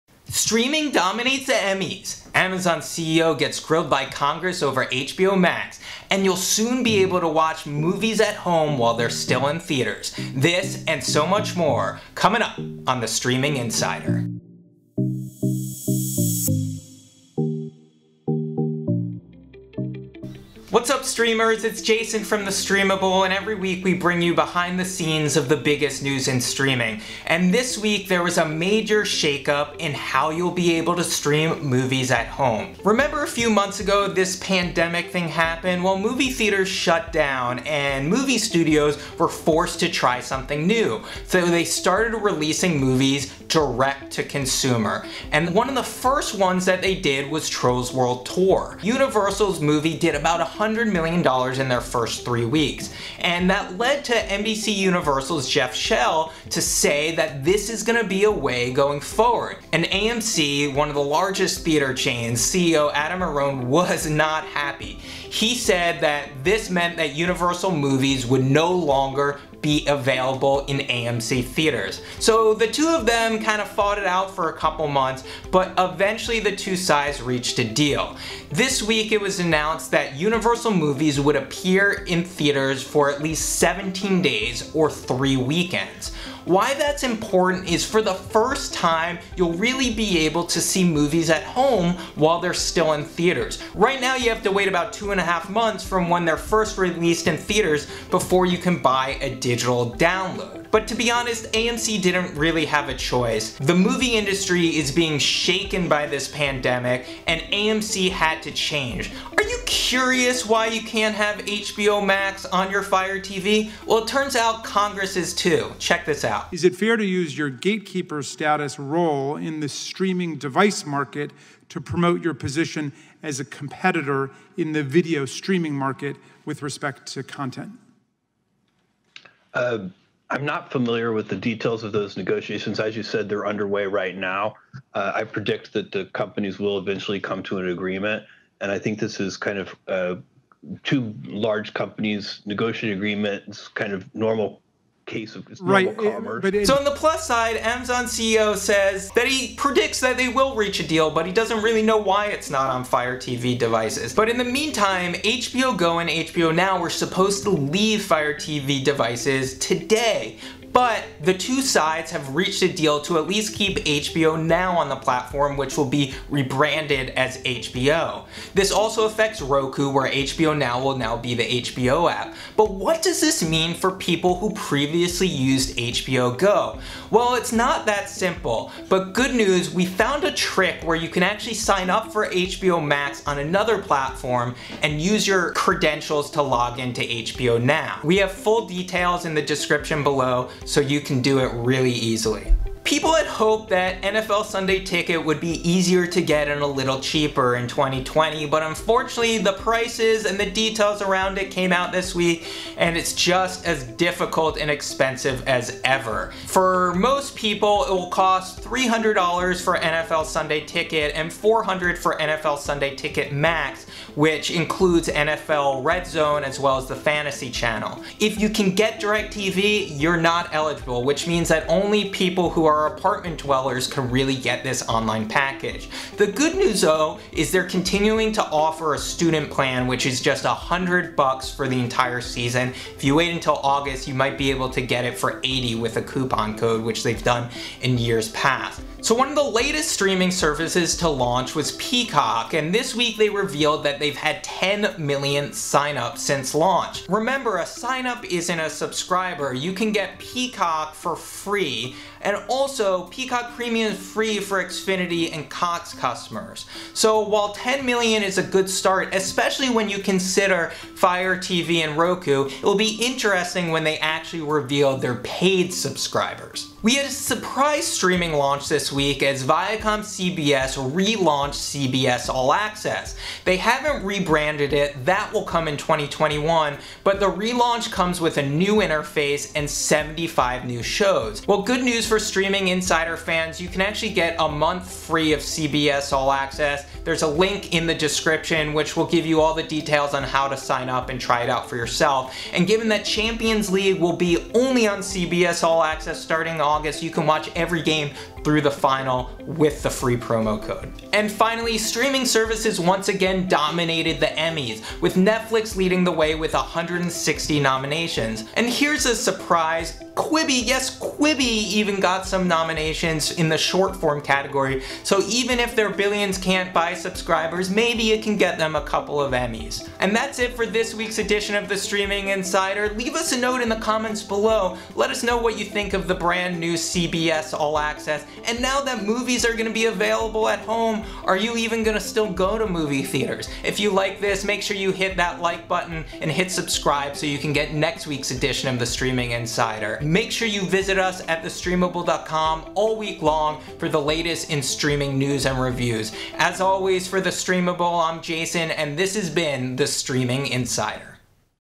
YOU CAN NOW STREAM IN-THEATER MOVIES AT HOME | The Streaming Insider (7/31/20)
New release movies are becoming streamable at home, Congress confronts Amazon CEO Jeff Bezos about HBO Max/FireTV deal, and Streaming Services dominate at the Emmys. Find out the latest news here at the Streaming Insider.

 1. AMC and Universal Reach Deal to Give 17 Day Theatrical Window to Films - :30

 2. HBO is Disappearing From FireTV - 1:50

How to Access HBO NOW with Your Cable & Satellite Credentials on Roku & Fire TV, After HBO Go Disappears:

3.. NFL Sunday Ticket is Still Expensive - 3:15

4. Peacock Announces 10 Million Sign-ups - 4:00

5. ViacomCBS Adds 70 Shows to CBS All-Access - 4:30
How to get one free month:

6. Streaming Services Dominate Emmy Nominations - 5:00